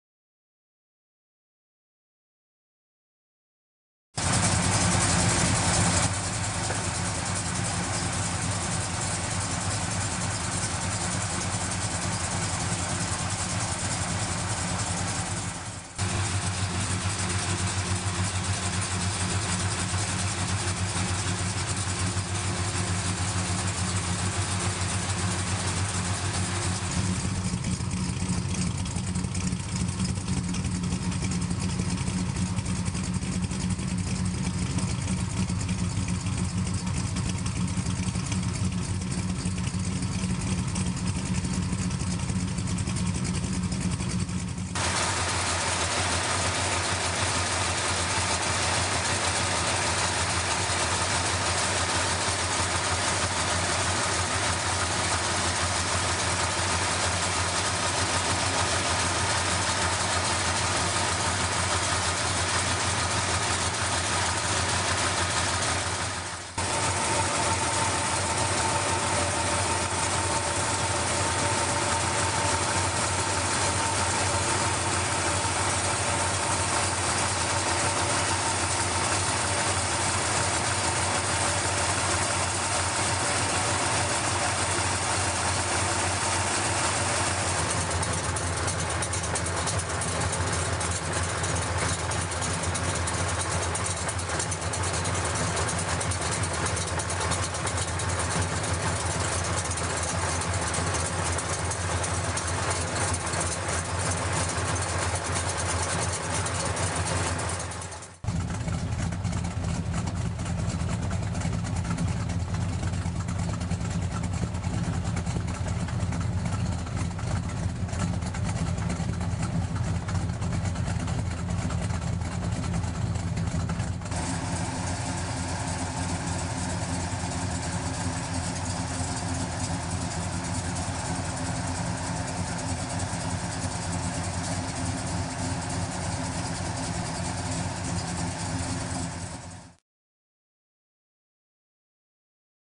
Bill's 1966 Plymouth Belvedere II
My Belvedere warming up in the driveway.